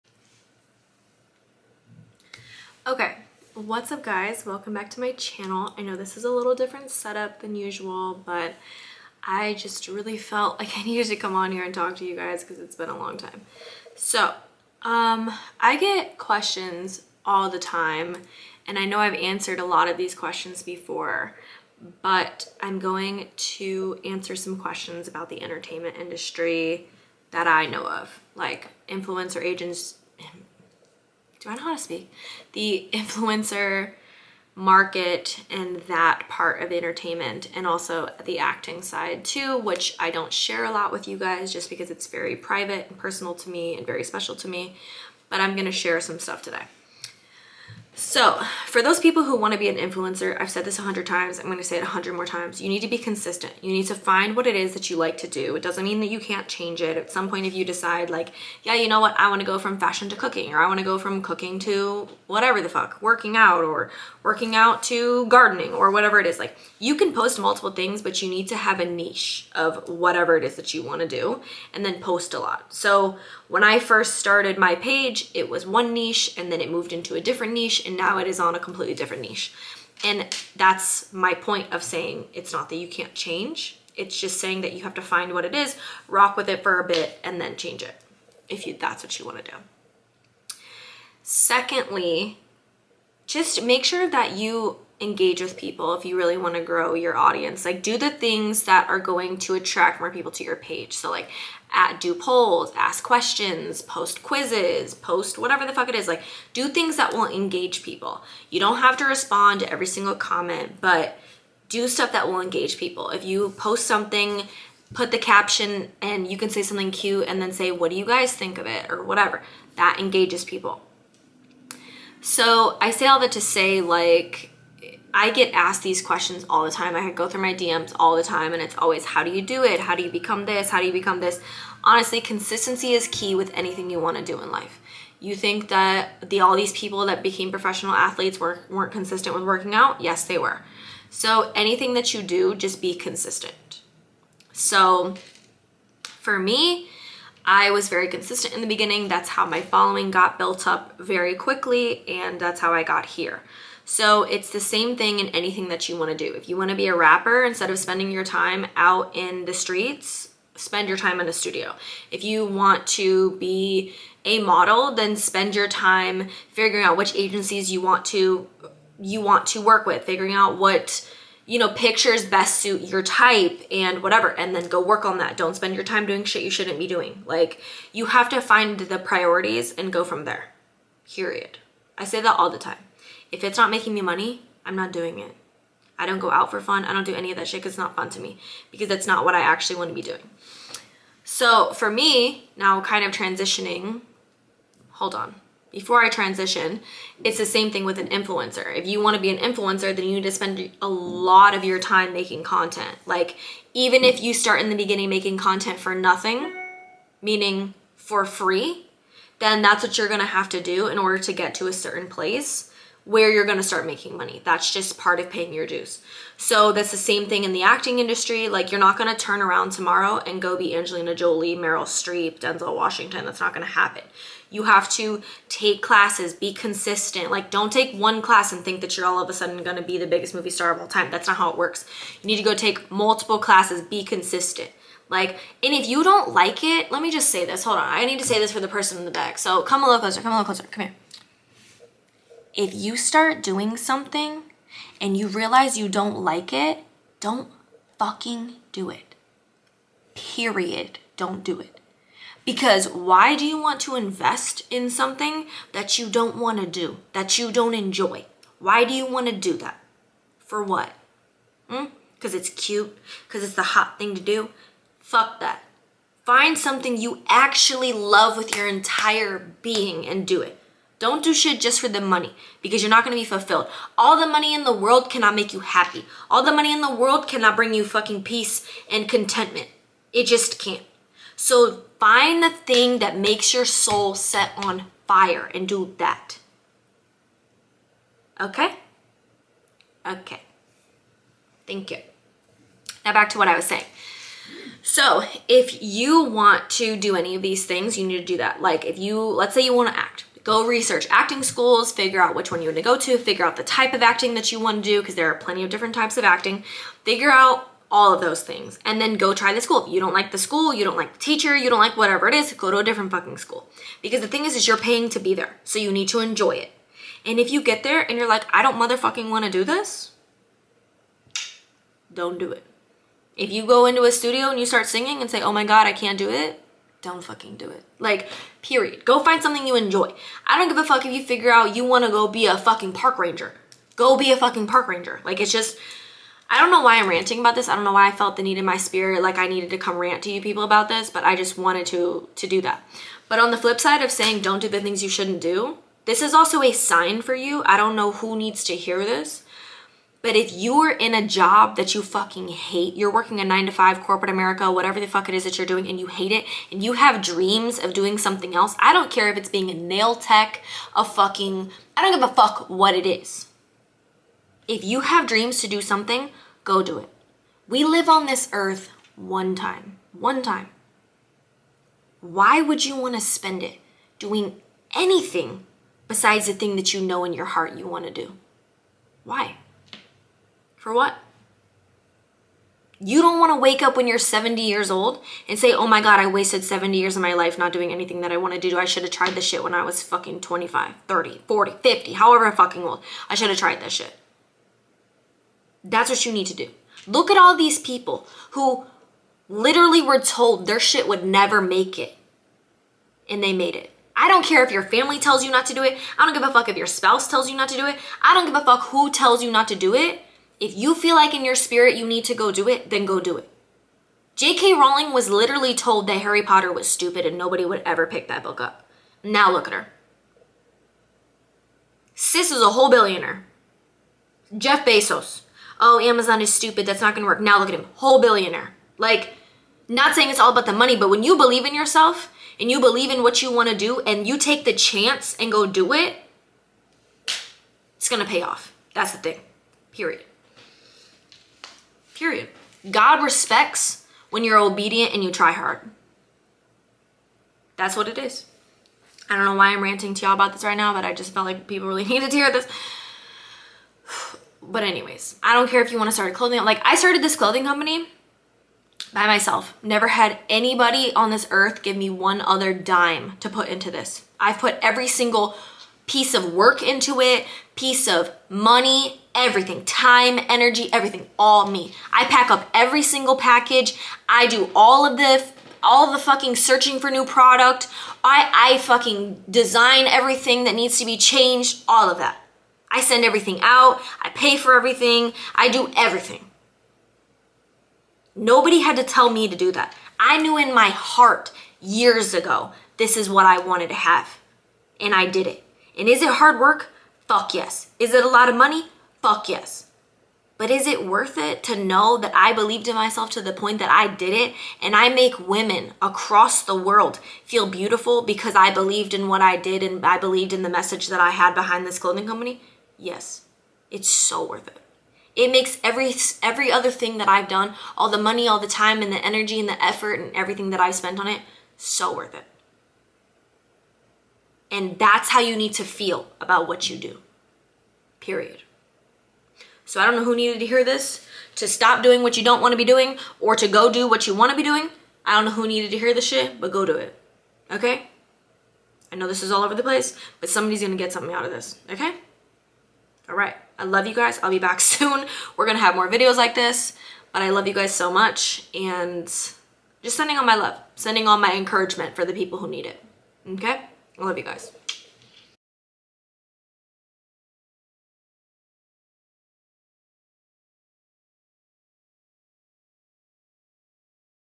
INSPIRATIONAL MESSAGE FROM P :) do what makes you happy - we only live on this earth once!!!
i don't know who needed to hear this but I pray that it is a blessing to you. sending all my love.
- P

IG, TikTok, Twitter: peytonfreestone